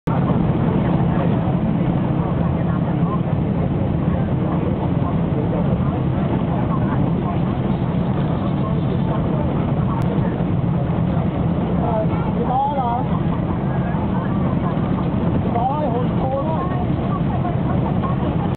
Le Cambodge vu d'un bus (2)
Trajet en bus entre Kompong Tom et Siem Reap